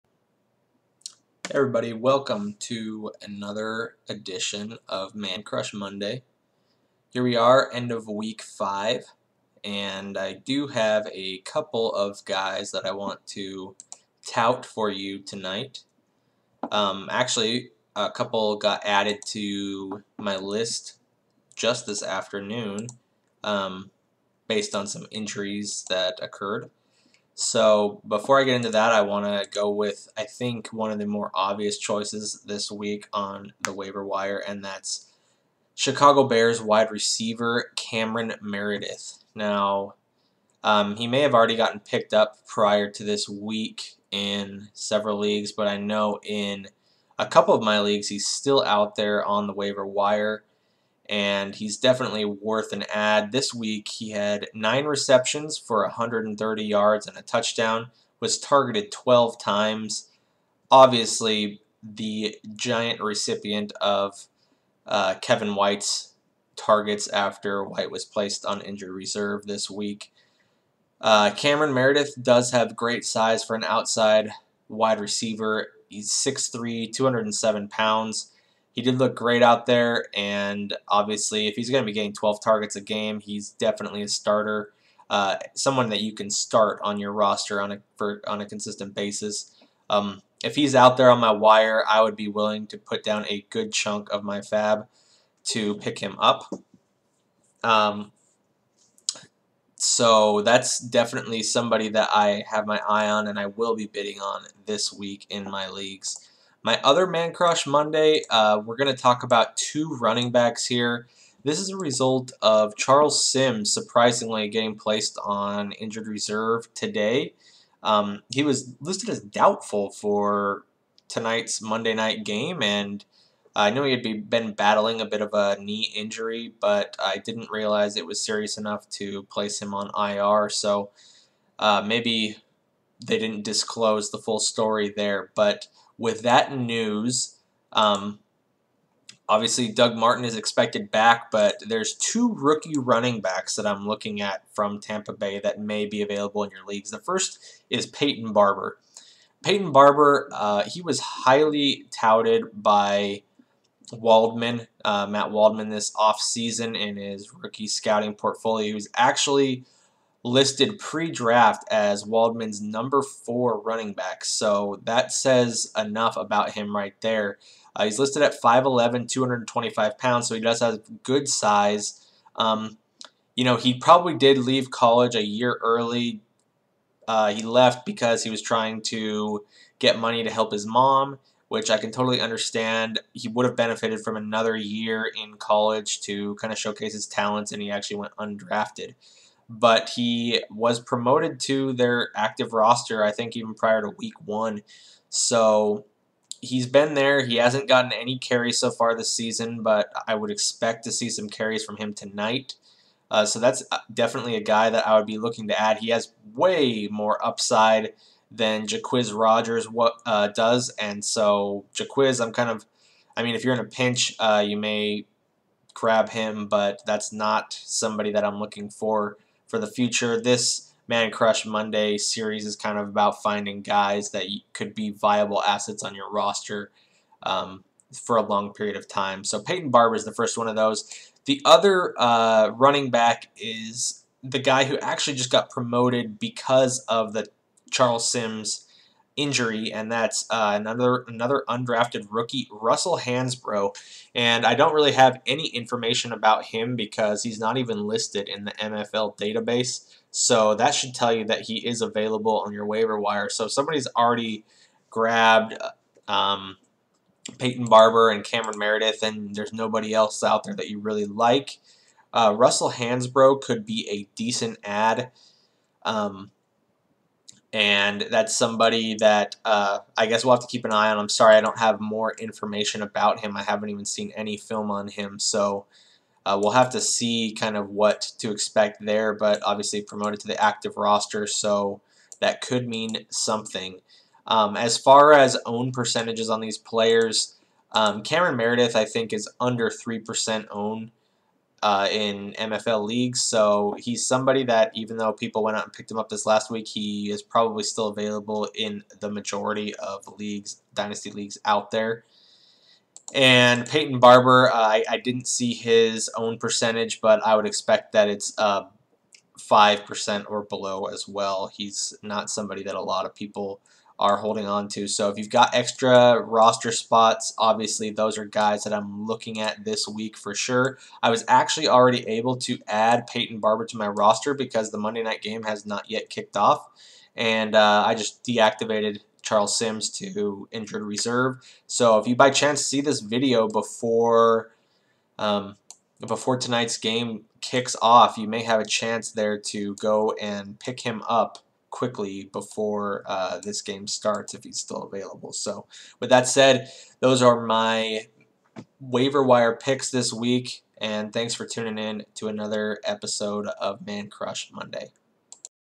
Man Crush Monday - Week 5
Week 5 - Cameron Meredith/Peyton Barber/Russell Hansbrough

The Man Crush Monday video series details ideal waiver wire targets in dynasty leagues.